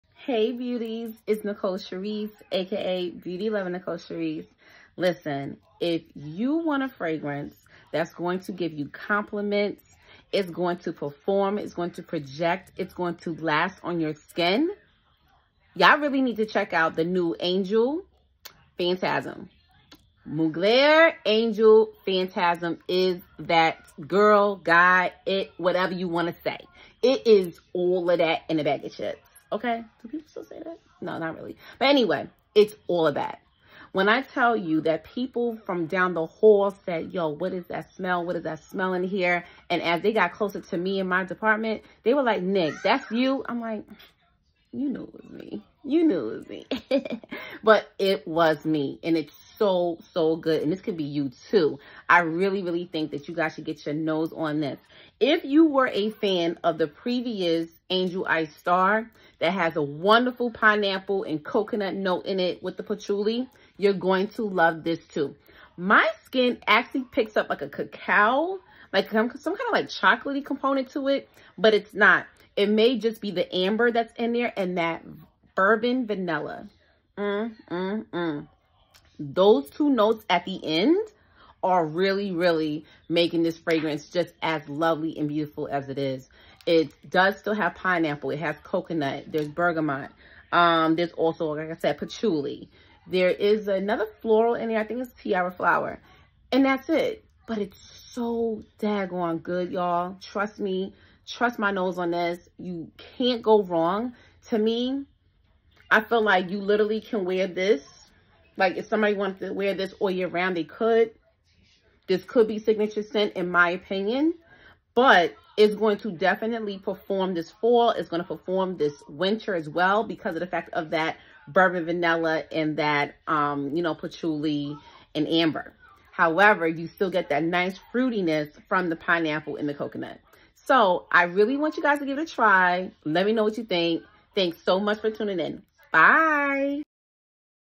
Angel Fantasm!!!!! Fragrance Review!!!!!
Hi beauties!!!

I had some successful blind buys recently and this was one of them!

@mugler Angel Fantasm is beautiful and I highly recommend it.